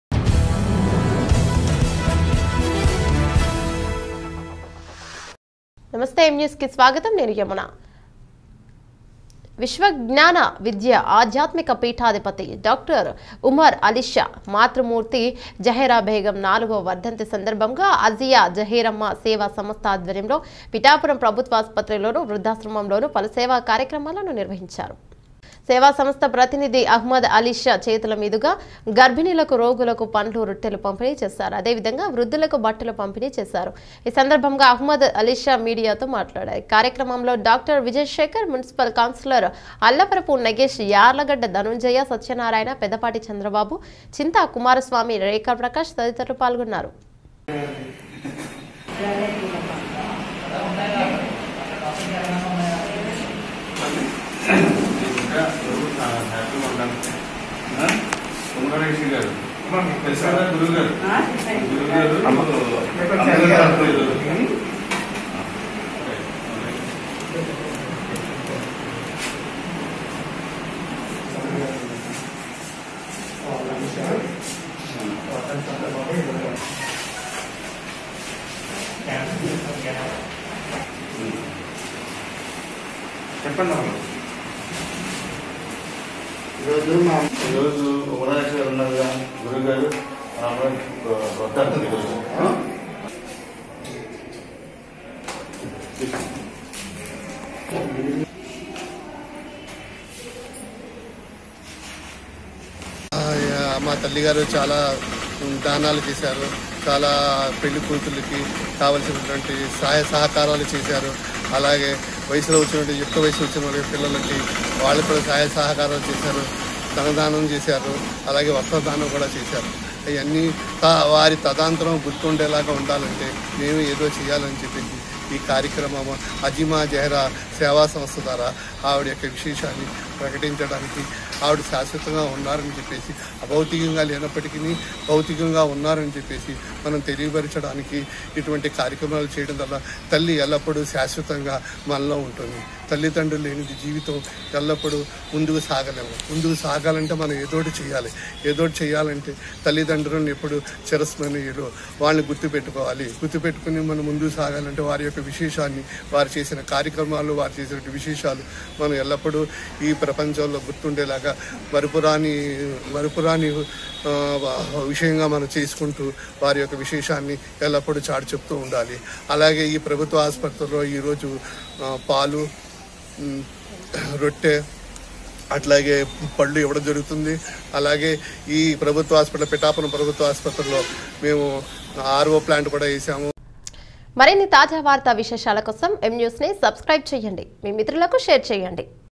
ఆధ్యాత్మిక పీఠాధిపతి డాక్టర్ ఉమర్ అలీషా మాతృమూర్తి జహేరాబేగం నాలుగో వర్ధంతి||M NEWS TELUGU
విశ్వవిజ్ఞాన విద్య,ఆధ్యాత్మిక పీఠాధిపతి డాక్టర్ ఉమర్ అలీషా మాతృమూర్తి  జహేరాబేగం నాలుగో వర్ధంతి సందర్భంగా అజియా జహేరమ్మ సేవాసంస్థ ఆధ్వర్యంలో పిఠాపురం ప్రభుత్వాసుపత్రిలోను,వృద్ధాశ్రమంలోను పలు సేవా కార్యక్రమాలు నిర్వహించారు.||M NEWS TELUGU